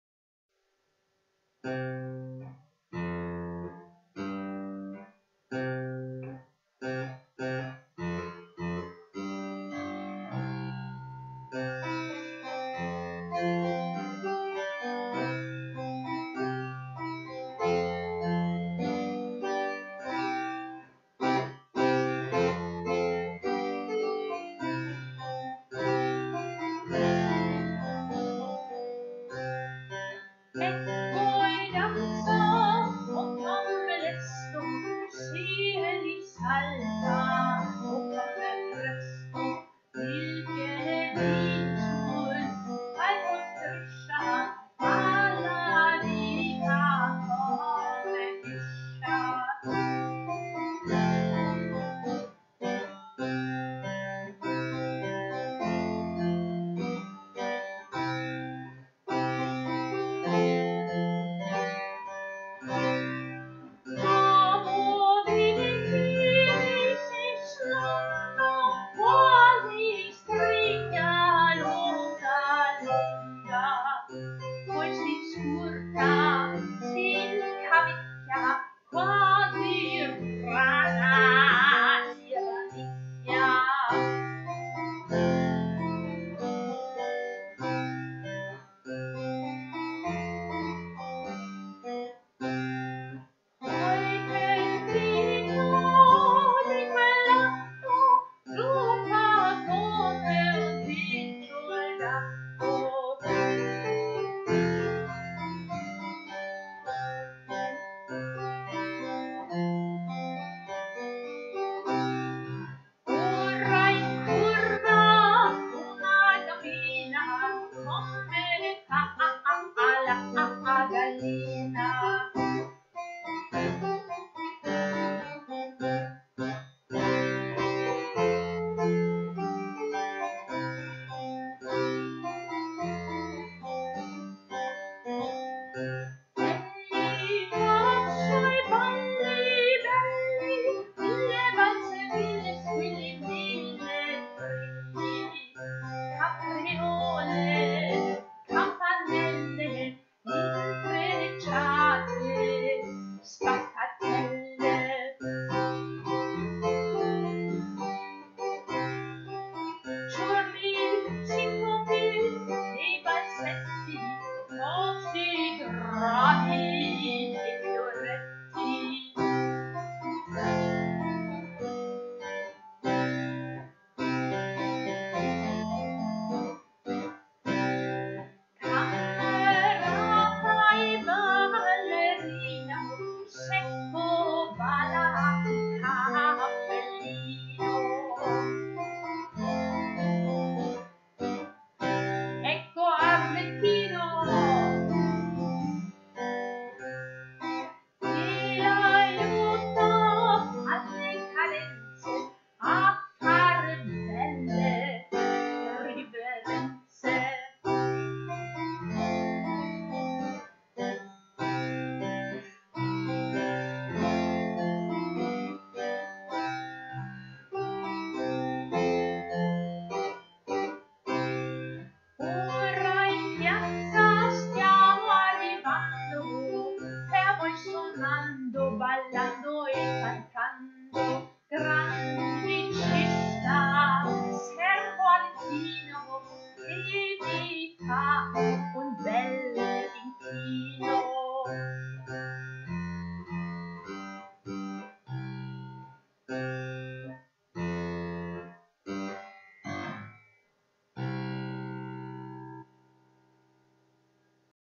Theatre Masks by Daniele Ruzzier
Handmade leather masks not only for the theatre all made by venetian actor Daniele Ruzzier - the workshop is based in Berlin, shipping throughout Europe!